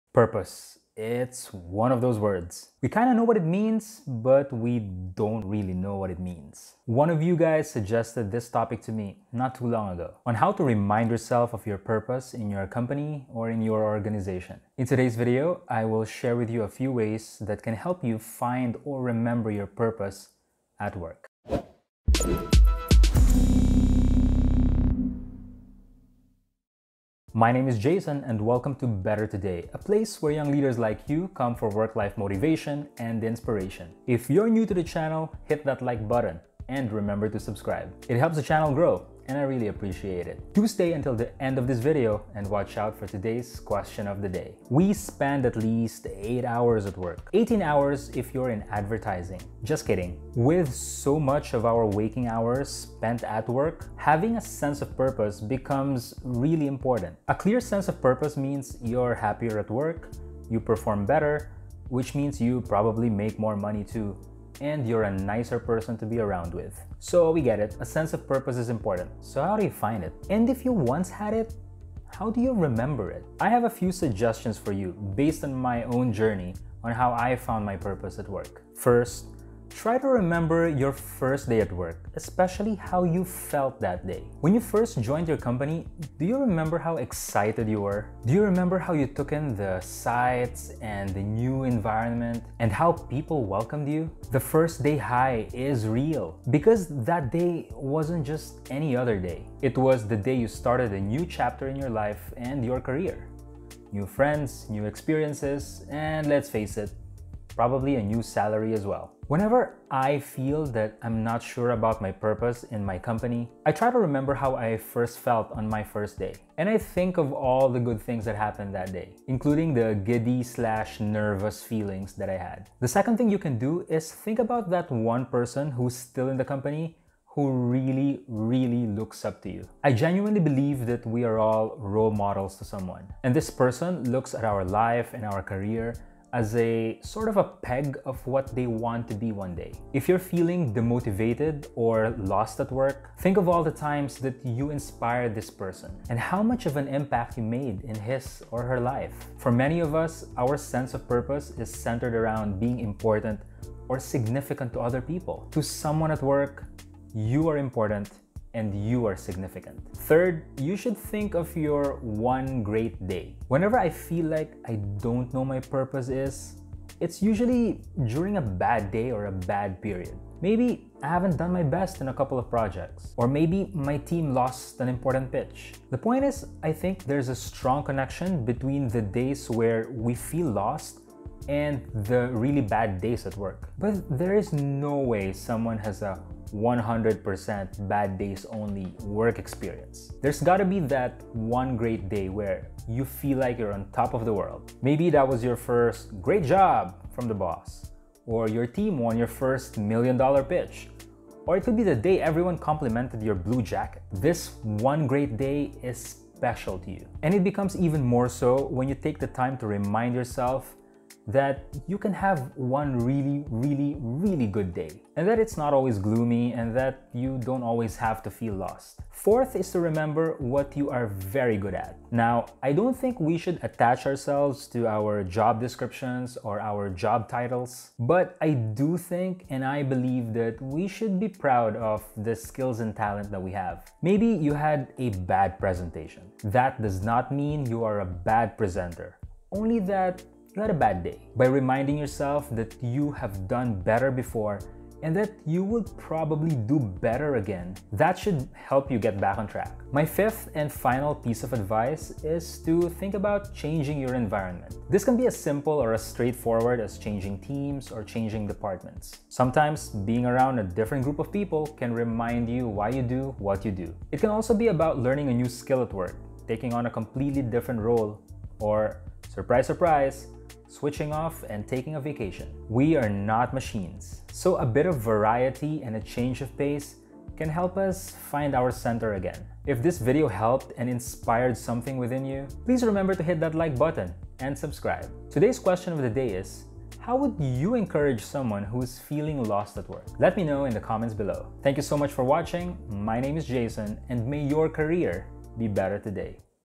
How to Find Your Sense of Purpose at Work (Again)
Over the last couple of months, I was asked by a few colleagues a pretty interesting question: how to find your sense of purpose at work (or in the workplace). This question made me look back over the last ten years - and list down what has helped me find a sense of purpose at work.

I cannot say 100% that I found my definitive purpose; I believe that we are always growing and changing and that our sense of purpose might change or evolve, too. However, I am quite fortunate to know that I DO have a sense of purpose - and that it keeps me going in an industry as challenging and as brutal as advertising.

Until then, I hope that your career - and your sense of motivation and purpose - is better today!

Thank you to my colleagues at ADA who tease me (in good spirits) about my YouTube channel. Lookie lookie, you guys are requesting videos now! Cheers, and I appreciate each and every one of you :)